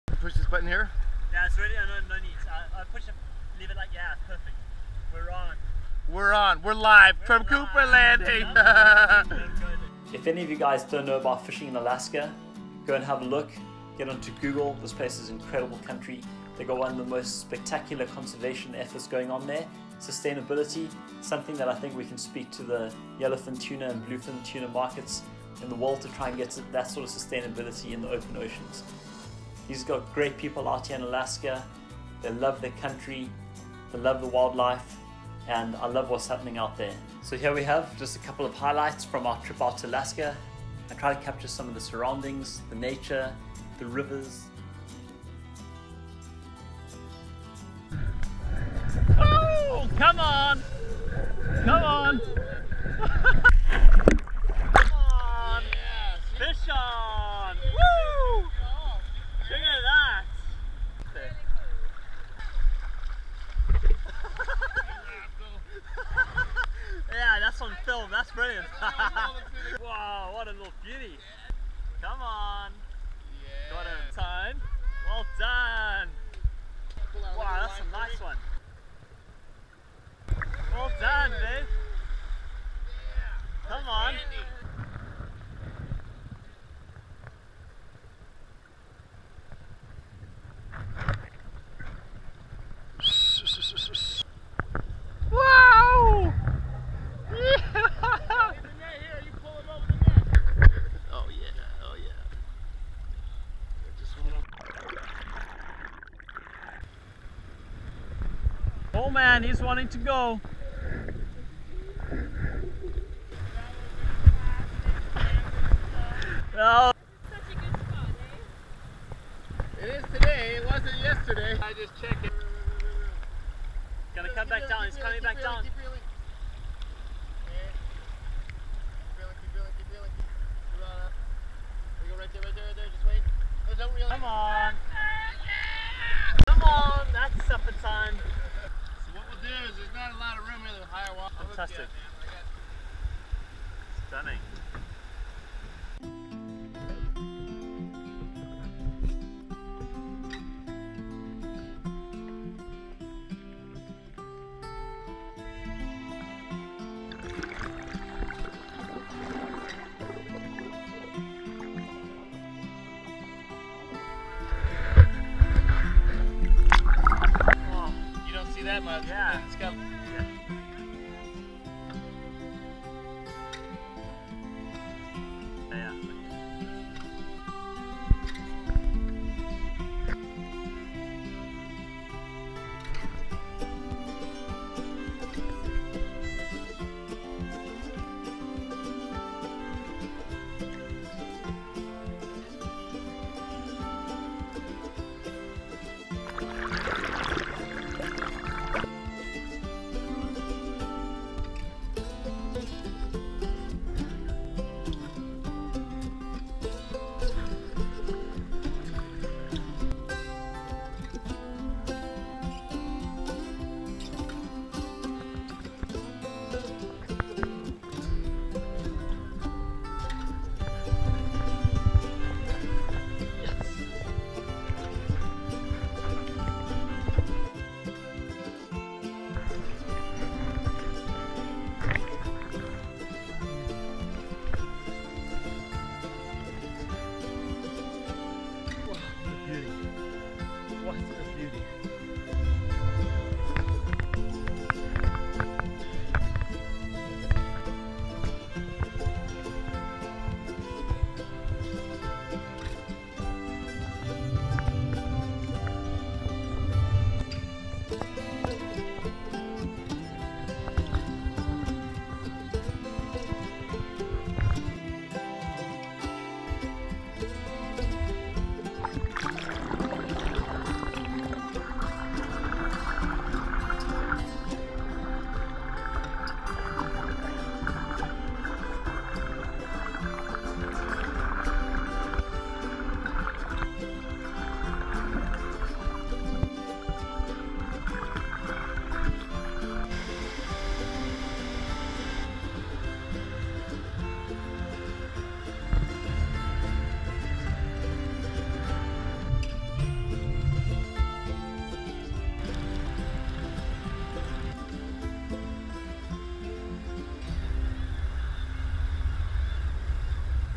SALMON RUN ALASKA - BIG FISH
Fishing the salmon run in Alaska, one of life's special places. The conservation effort by the team in Alaska is something that I hope can spread across to other regions of the world. There is an abundance of fish, and the system in places, allows this to be so year after year. Cold water, clean water, fresh fish!